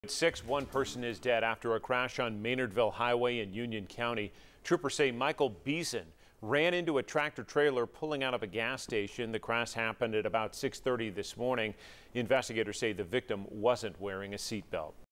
Person dead after crash on Maynardville Highway in Union County
Troopers said Michael Beason ran into a tractor-trailer while pulling out of a gas station.